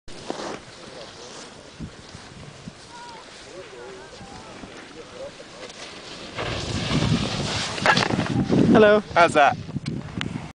Skiing in Tignes, France.
Showing off traversing skills, albeit looking like I am riding a horse.